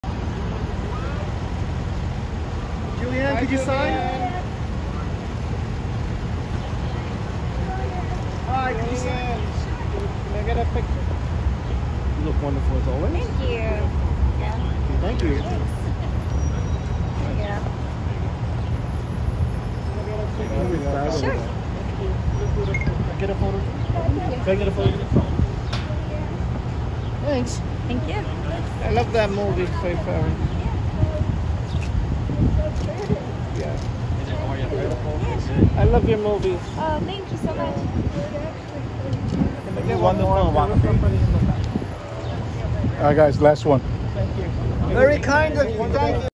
Julianne Hough meets Fans outside "The View"
Julianne Hough signing autographs and posing with Fans after her appearance on "The View" ABC TV show